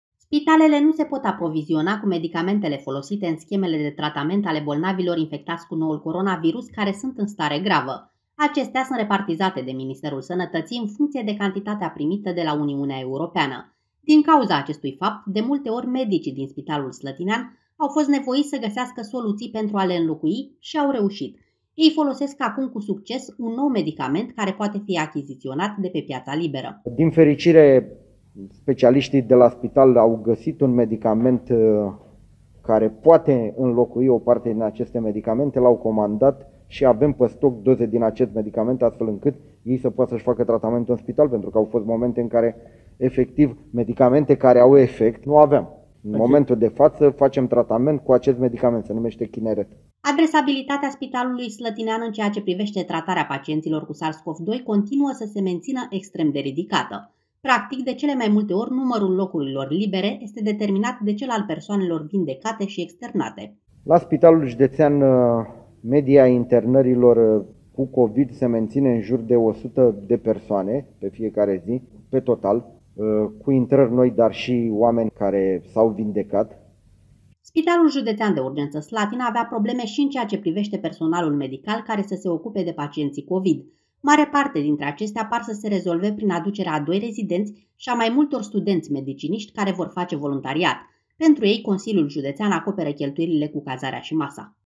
Medicii slătineni, soluții pentru lipsa medicamentelor pentru COVID
Aprovizionarea  spitalelor cu principalele medicamente folosite în tratarea pacienților în stare gravă, infectați cu COVID, continuă să fie deficitară. Pentru că numărul dozelor a fost permanent mult mai mic decât al celor necesare, medicii slătineni au încercat să găsească soluții de înlocuire a acestora și au reușit. Ei folosesc acum, cu succes, un alt medicamet ce poate fi achiziționat la liber de către unitatea medicală. Din dorința de a avea cu ce să-i trateze pe pacienții internați care se află în stare gravă, unitatea medicală a făcut deja o comandă importantă.